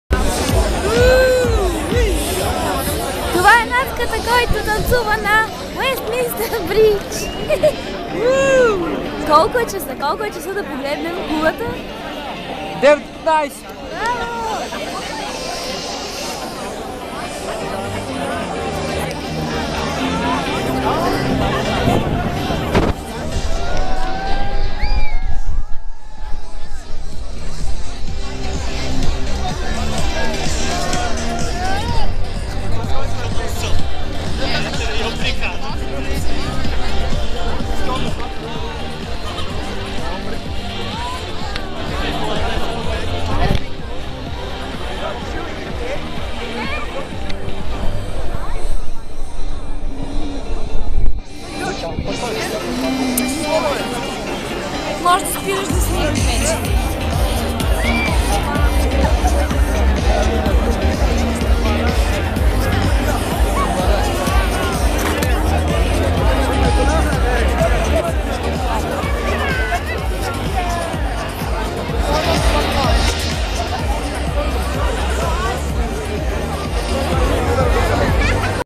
This is how we greeted the year of 2013 - party on the Westminster bridge, London
party, dance, Happy New Year!